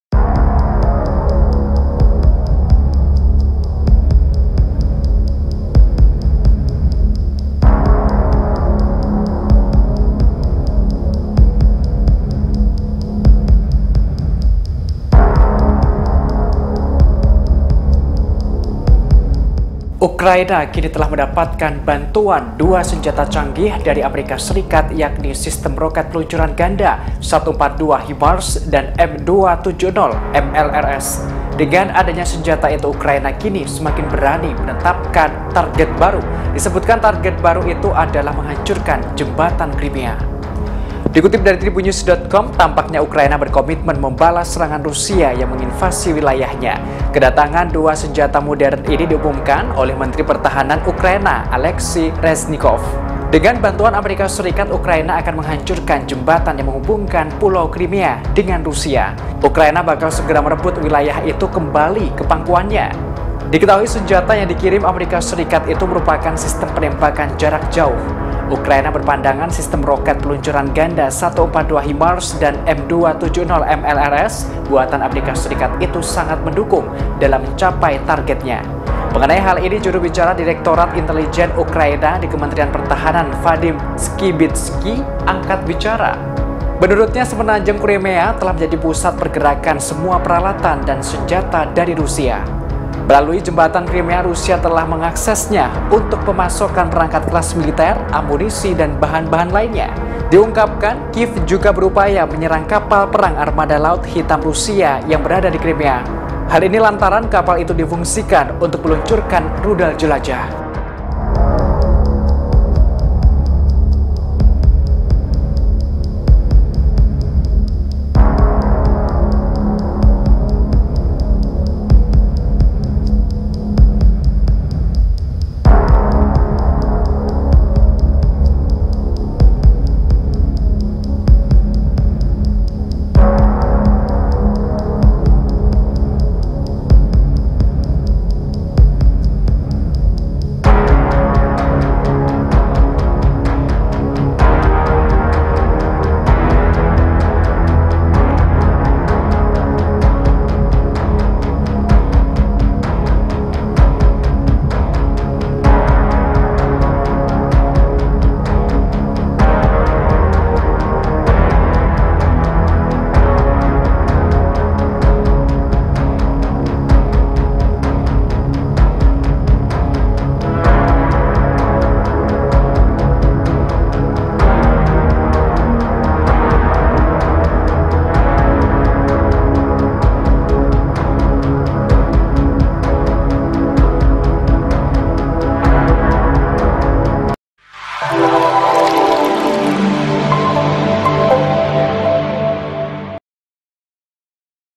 Amerika Kirim 2 Jenis Senjata Canggih ke Ukraina, 142 HIMARS dan M270 MLRS
Amerika Serikat mengirimkan dua senjata canggih yang baru pada Ukraina.

Dua senjata canggih tersebut yakni sistem roket peluncuran ganda 142 HIMARS dan M270 MLRS.

Didukung oleh senjata itu, maka Ukraina kini semakin berani untuk menentukan target baru